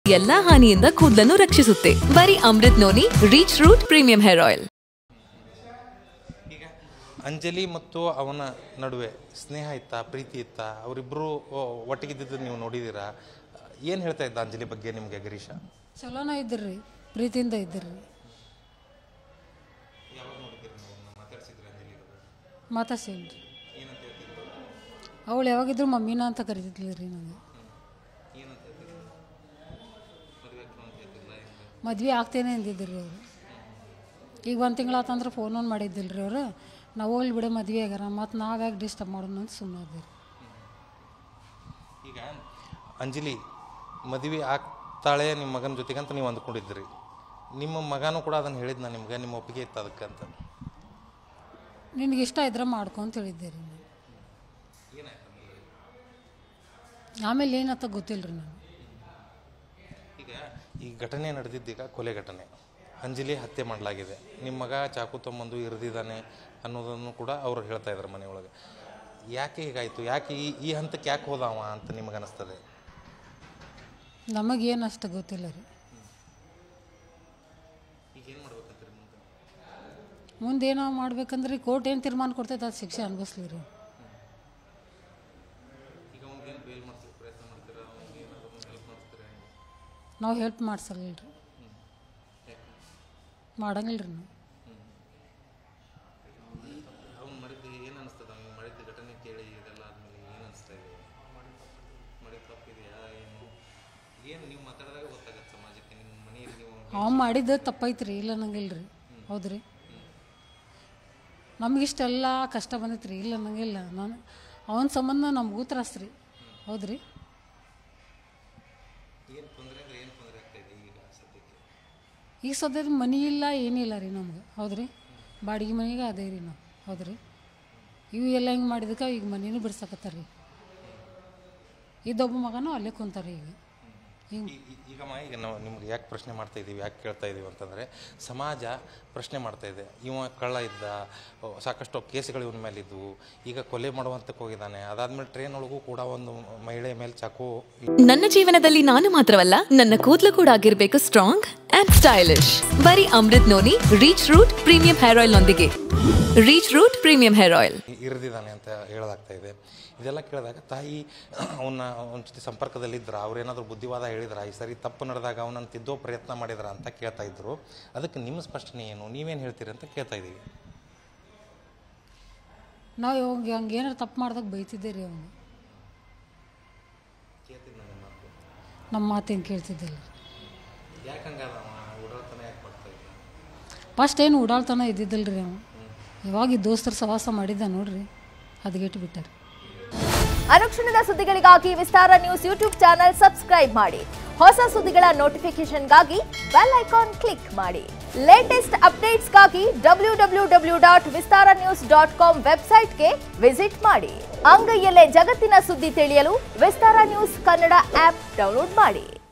Girish Mother First Reaction On Hubballi Case | Anjali | ಅಂಜಲಿ ನನ್ನ ಪ್ರೀತಿಯಿಂದ ಮಮ್ಮಿ ಅಂತಿದ್ಲು..!
Girish Mother First Reaction On Hubballi Case | Anjali Incident | ಅಂಜಲಿ ನನ್ನ ಪ್ರೀತಿಯಿಂದ ಮಮ್ಮಿ ಅಂತಿದ್ಲು..!